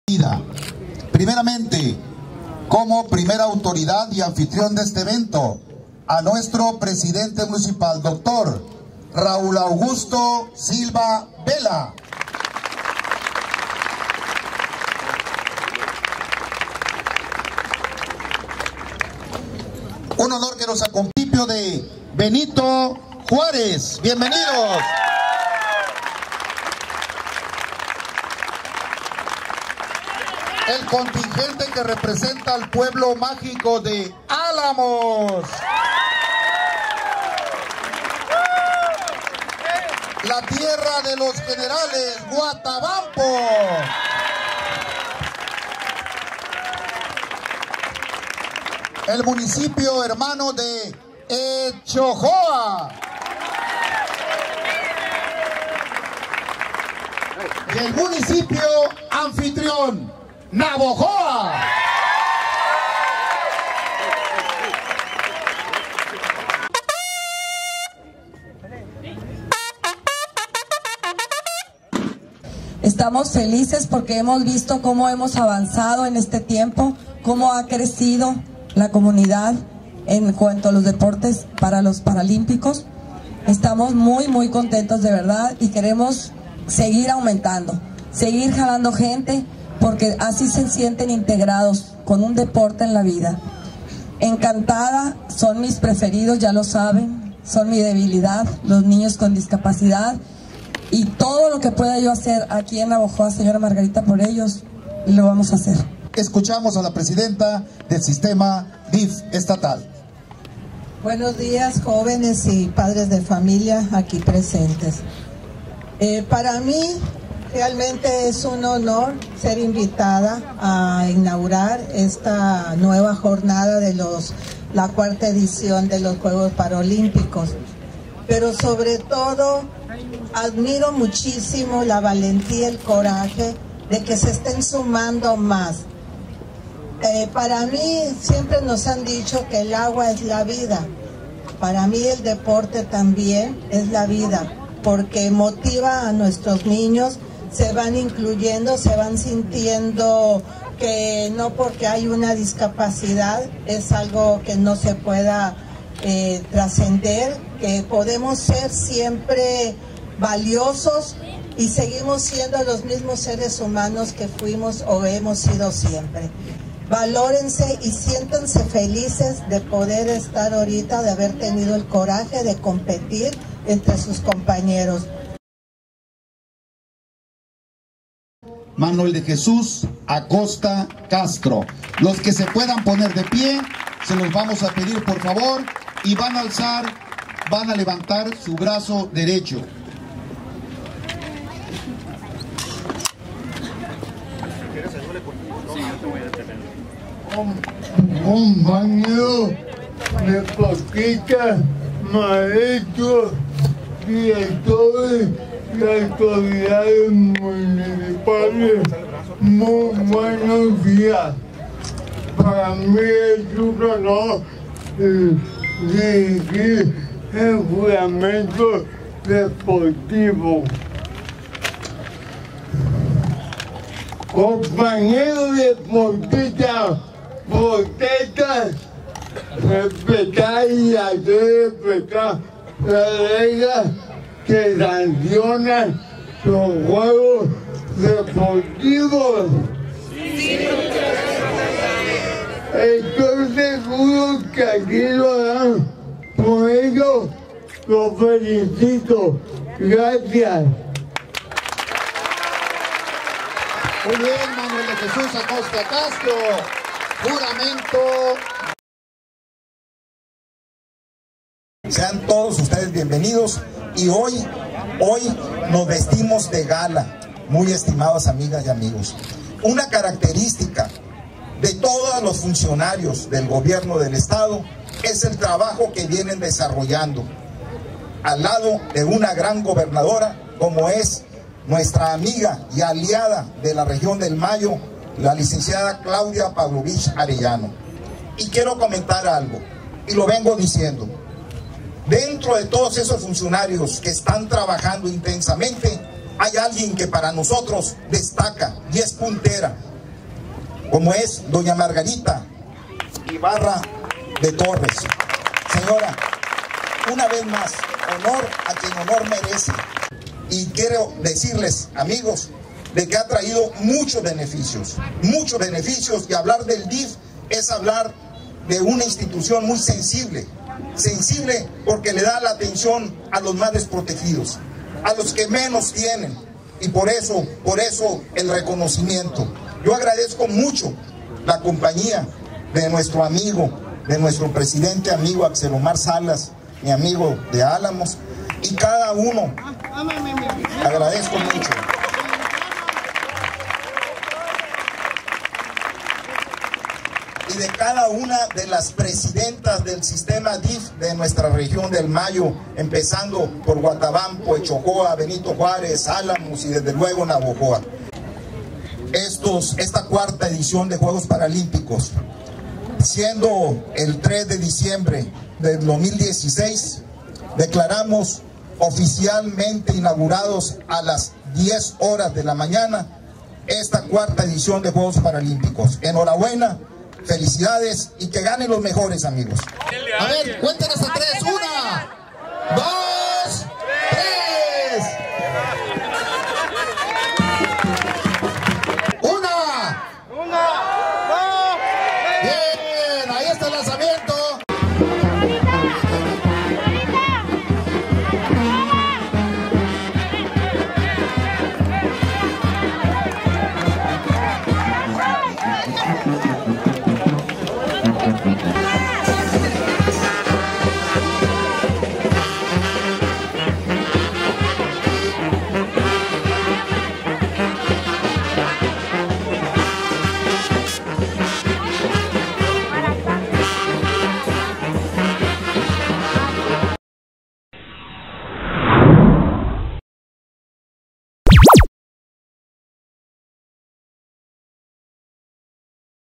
4tos. Juegos Paralimpicos
Para continuar fortaleciendo el entorno de las personas con discapacidad y tengan una mayor integración con la sociedad, el Gobierno Municipal, en coordinación con el Sistema para el Desarrollo Integral de la Familia (DIF Estatal y Municipal), inauguraron los "Cuartos Juegos Paralímpicos Regionales del Sur de Sonora", donde se presentaron más de 150 atletas representando los municipios de Navojoa, Huatabampo, Benito Juárez, Etchojoa y Álamos.